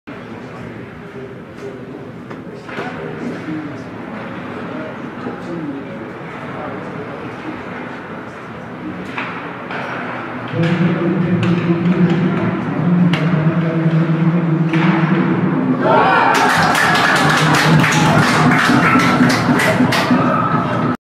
Реализация большинства. ХК Динамо 2005 -ХК Витязь 2005
Важный гол в матче против ХК Динамо во главе с сильным тренером А.А. Корредором